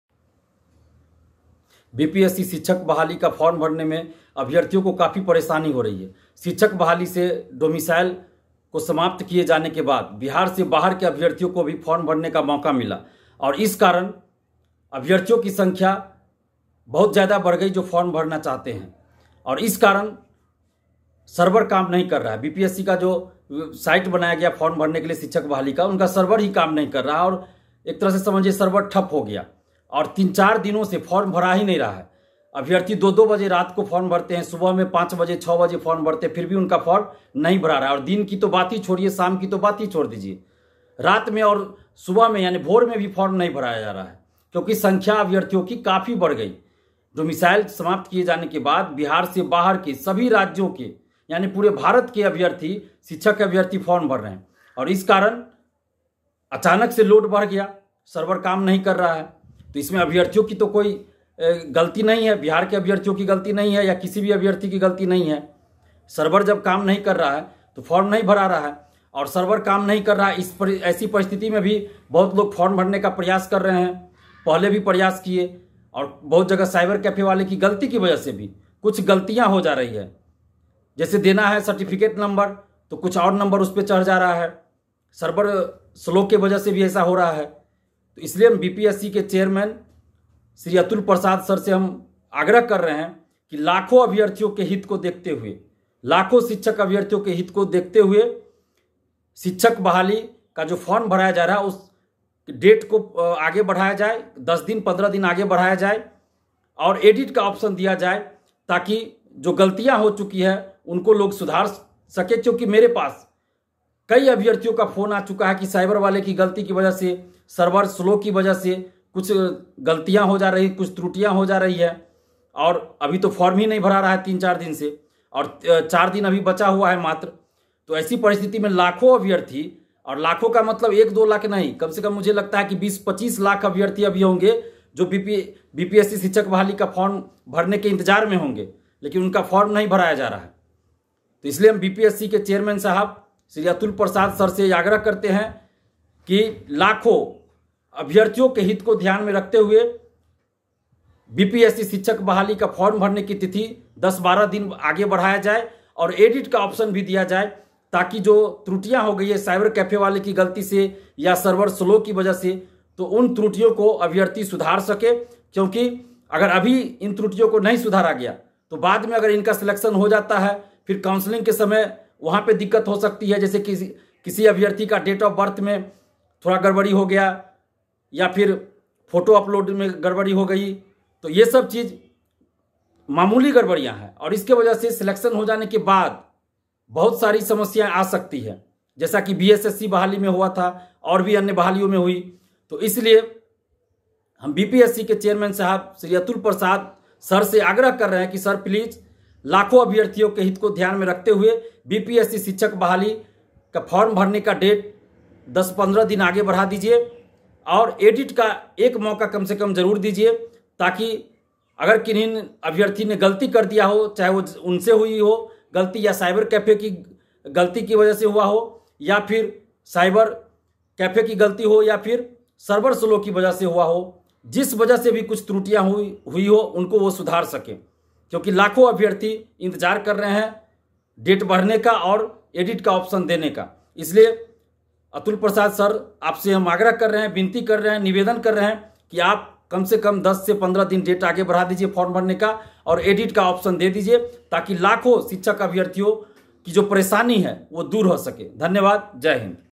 Bihar Teacher Recruitment : BPSC का सर्वर डाउन, 'फार्म भरने की तिथि बढ़ाई जाए' - छात्र नेता की मांग
छात्र नेता दिलीप ने शिक्षक बहाली में फार्म भरने की तिथि बढ़ाने की मांग की है. कहा कि सर्वर डाउन होने से अभ्यर्थियों को परेशानी हो रही है. सर्वन डाउन होने से जानकारी गलत भरा जा रहा है. इसलिए विभाग इसे दुरुस्त करने का काम करें.